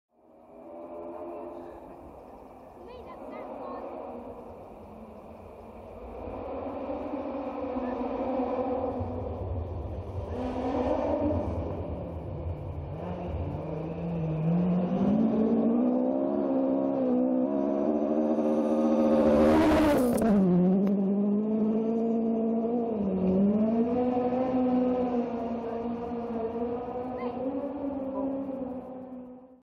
Marty McCormack/Phil Clarke - Neath Valley Stages 2013
A quick clip of McCormack's brilliant sounding Mk2 Escort.